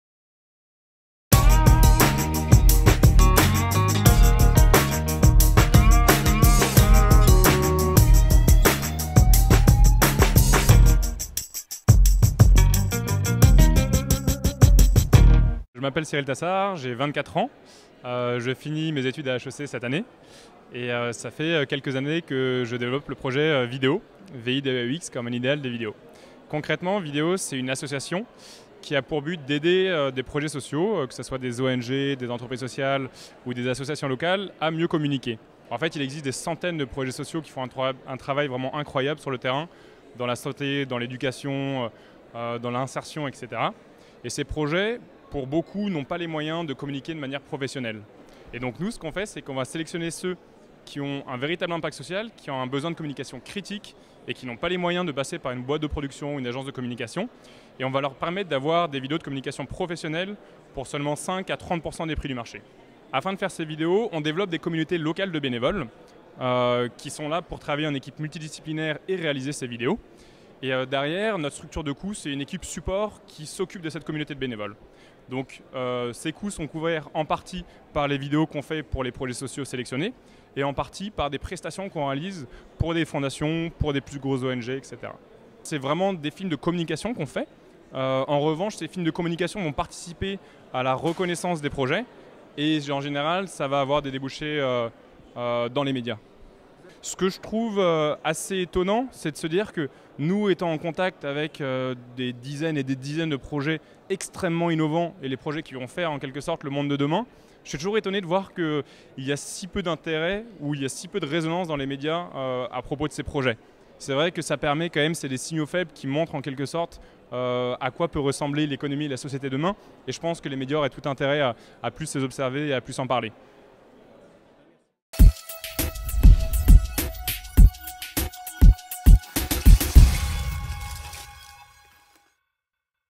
Vidéaux, l'agence vidéo qui aide les associations à communiquer
Une agence vidéo pour les associations, les entreprises sociales et les ONG ? C'est le créneau qu'a choisi d'occuper Vidéaux, une association née en 2007 et fermement décidée à soutenir les projets sociaux, explique son cofondateur Cyril Tassart, tout droit sorti d'HEC, dans cette interview accordée à Mediatico (...)